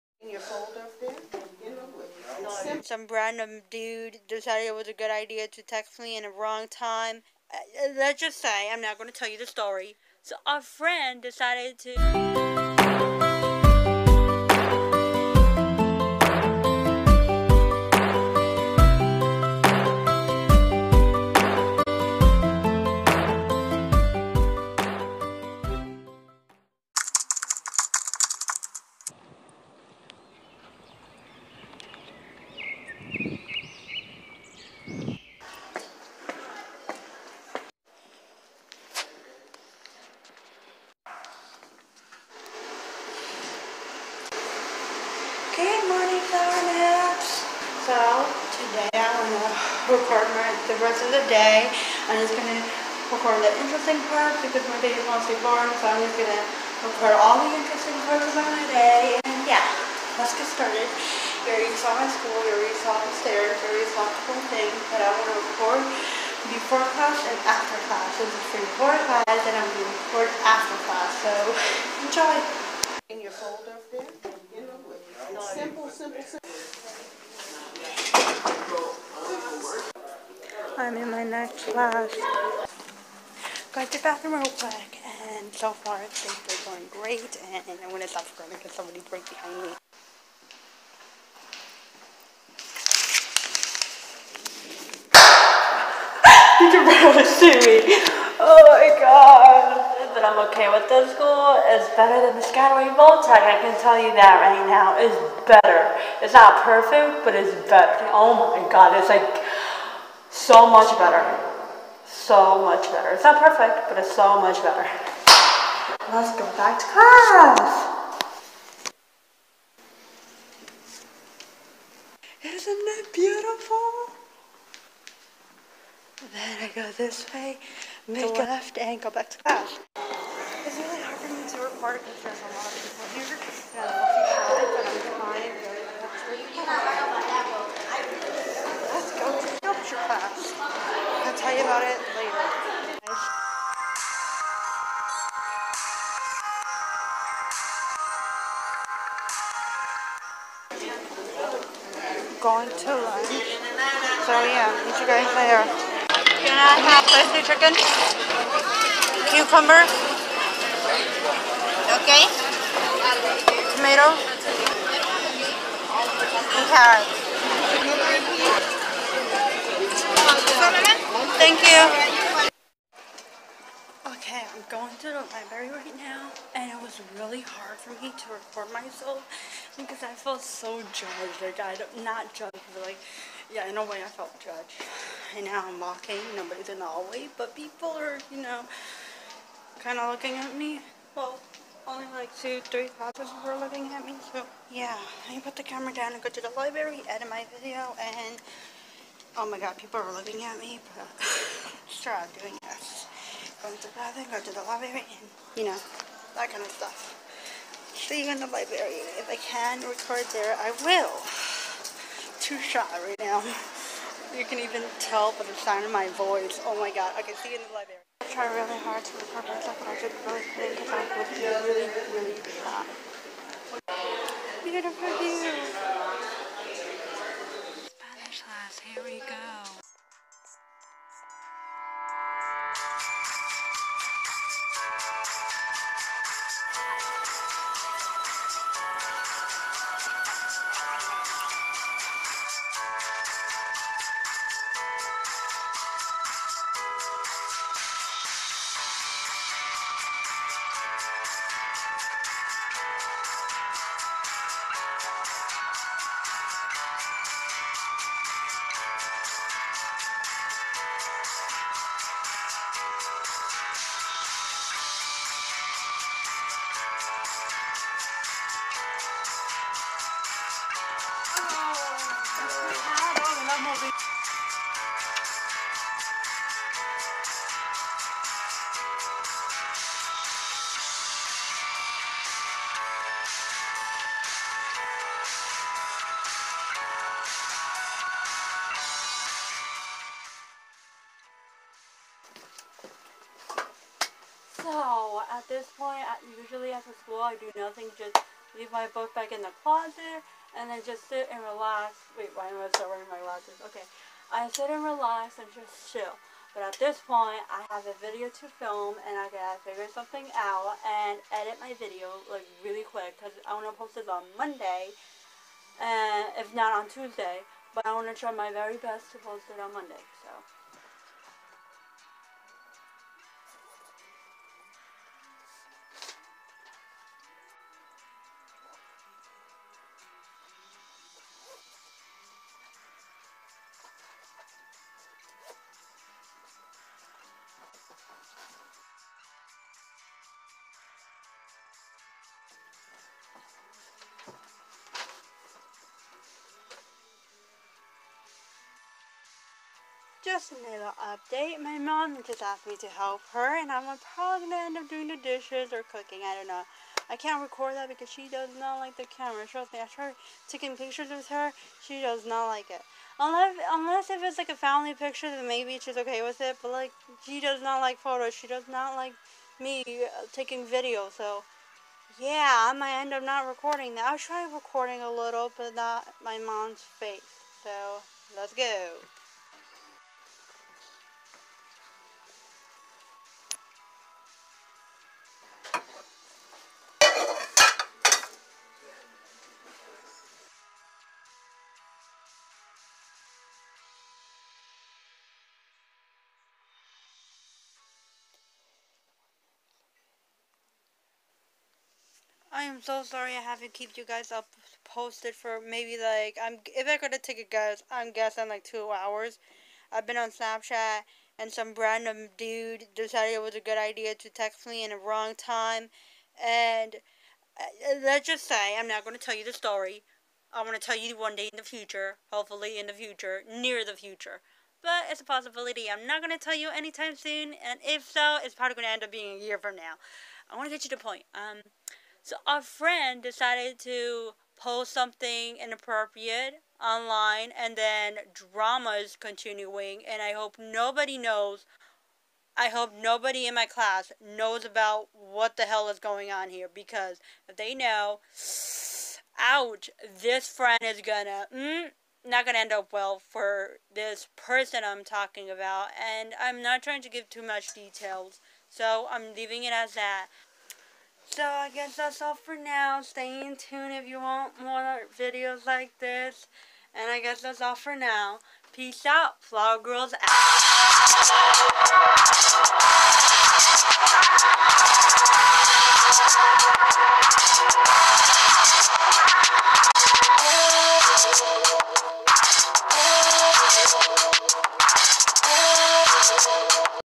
A Day In My life Vlog Challenge! (I Tried)
Hey Flower Nest, I was going to post this around morning but the Wi-Fi was too slow! Now you know how is like a day in my life! Is not mushed to show, at school I don't even spend time with my friends they'd rather ditch me then spend time with me!

There is only one true friend I tried spending time with her as much as I can, it's sad me and her don't have classes together.

Do you want to see my challenge videos click down below!